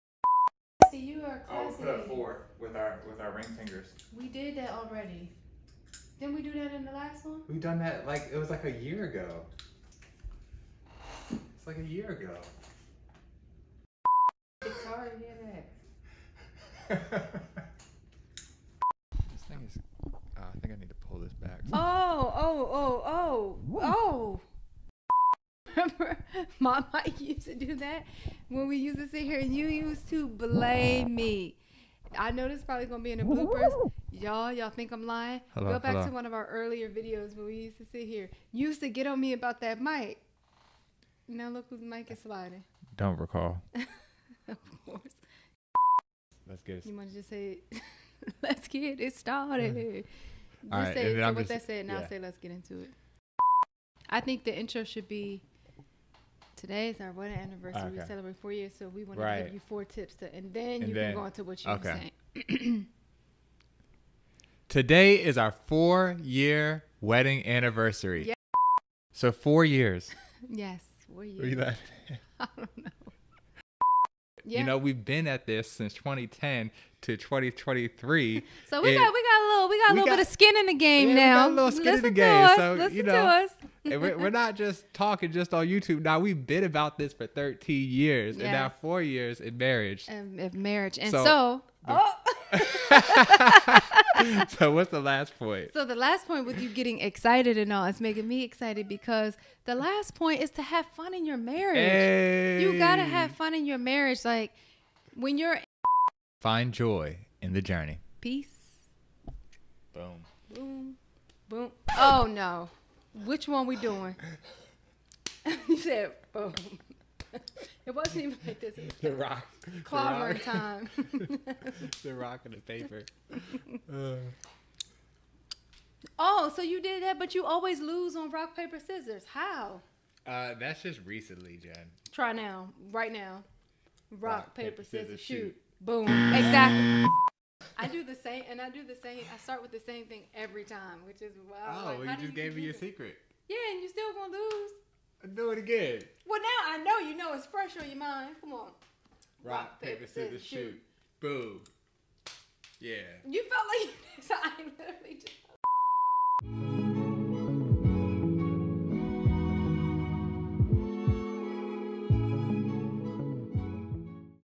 EPISODE BLOOPERS - 4 MARRIAGE TIPS to Keep the Spark Alive in your Relationship!
BLOOPERS EDITION FROM EPISODE - (keeping it fun with laughs)

-4 years ago today, WE GOT MARRIED! To celebrate 4 year anniversary, we provide 4 Marriage Tips that have helped us keep the SPARK ALIVE in our Relationship over the last 13 overall years! Let us know if you found some value in this conversation for you!